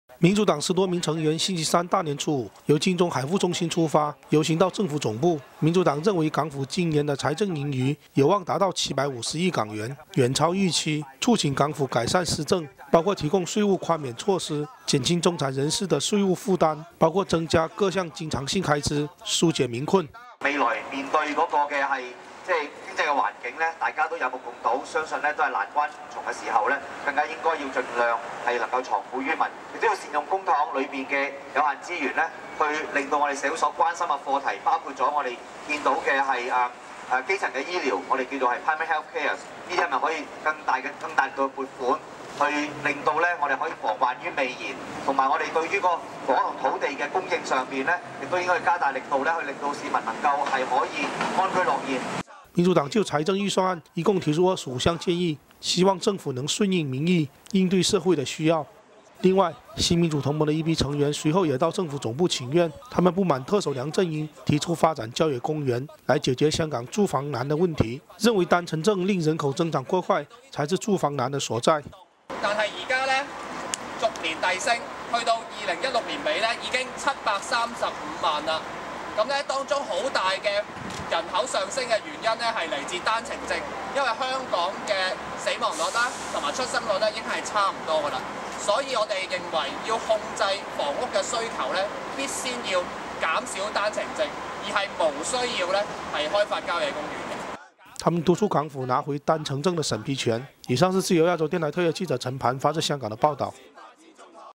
香港政党促港府善用财政盈余 纾解民困
香港早前发布了新的施政报告，本月底又将公布新一季度的财政预算案，预计将有700多亿港元的财政盈余。有政党在新年期间请愿，要求港府改善施政纾解民困。自由亚洲电台特约记者陈槃报道。

听众谈话